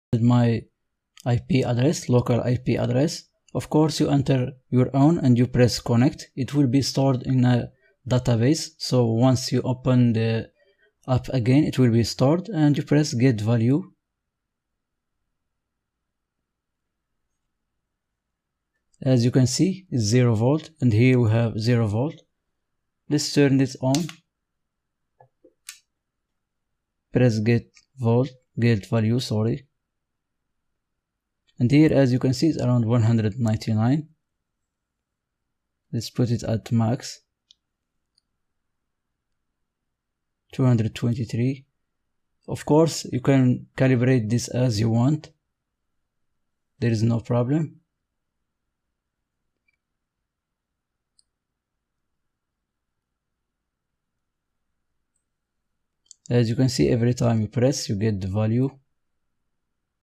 ESP8266 WiFi voltmeter with android app inventor and arduino
Make an ESP8266 12E (NodeMcu) voltmeter up to 250VAC and read it with Arduino phone by creating an App from MIT App Inventor. The tutorial has also Adafruit MQTT